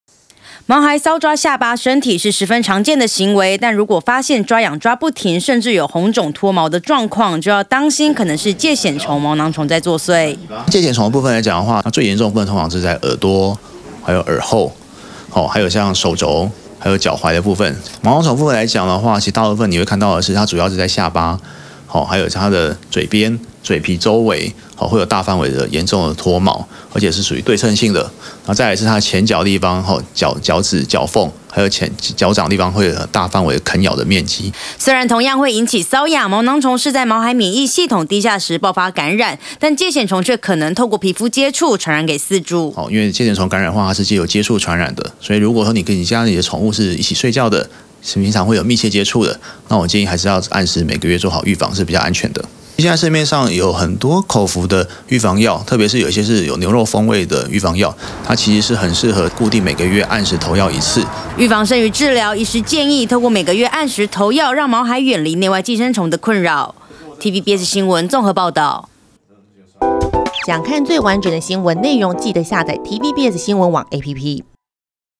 毛孩抓癢抓不停 恐是疥癬蟲.毛囊蟲作祟｜TVBS新聞
夏天高溫潮濕，加上寄生蟲猖獗，毛孩可能會染上皮膚病，醫師提醒，如果發現狗狗不斷抓癢，皮膚出現紅腫、脫毛，就要特別注意。